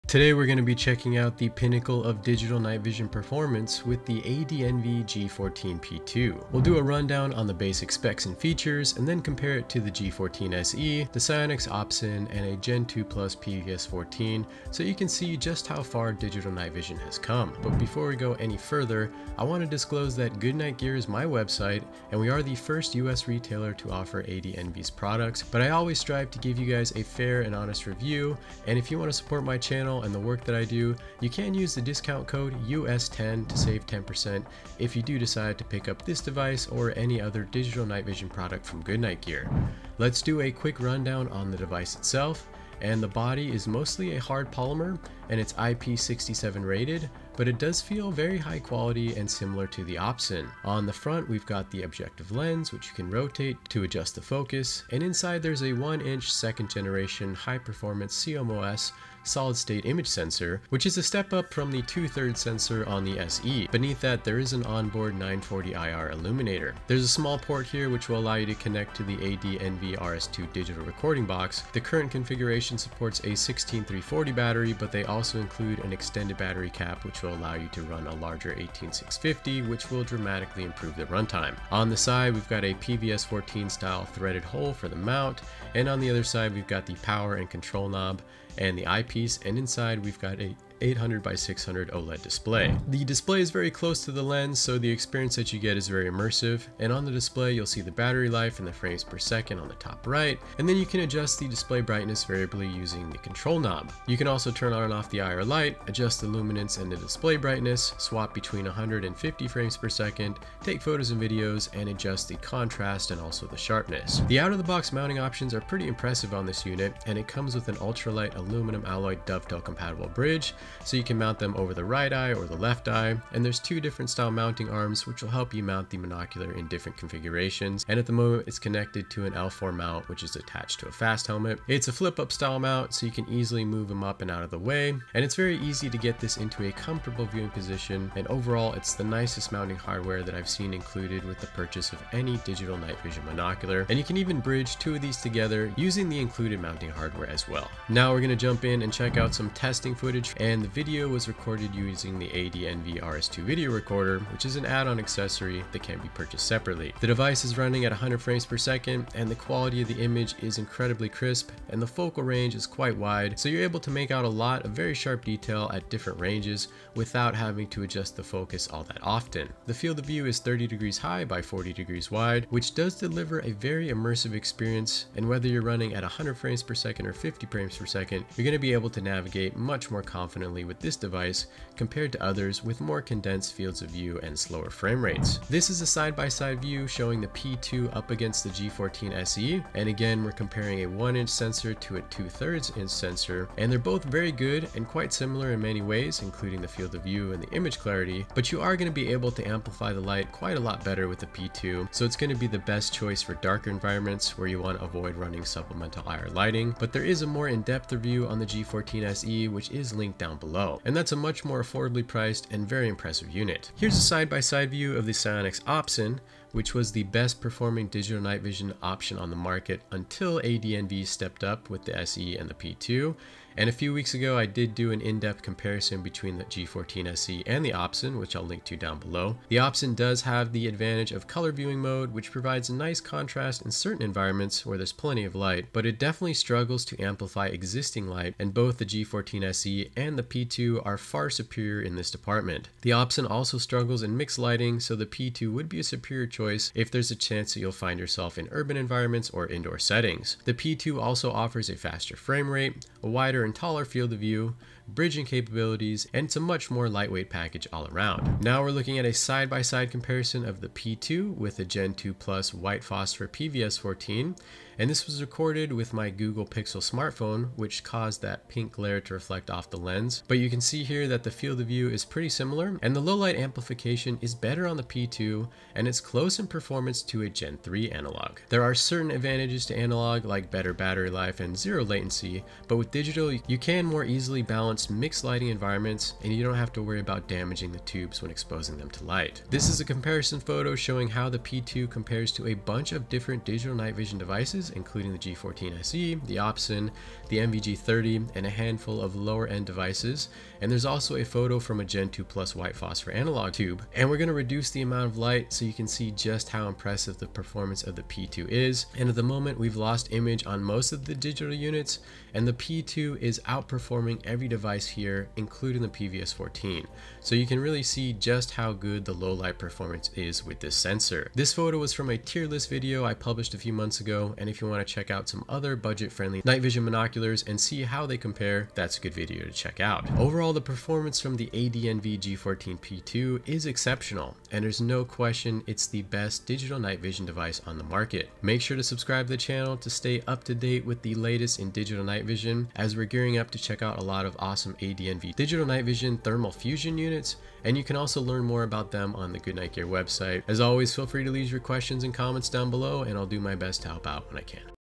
The Best Digital Night Vision 🌖 ADNV G14P2
Timestamps
00:00 Intro
00:39 ADNV G14P2 Tabletop Overview
03:05 G14P2 vs G14SE
03:33 G14P2 vis Sionyx Opsin
04:18 G14P2 vs Gen 2+ Analog
04:49 Night Vison Comparison

Thanks to  @WhiteBatAudio  for the music!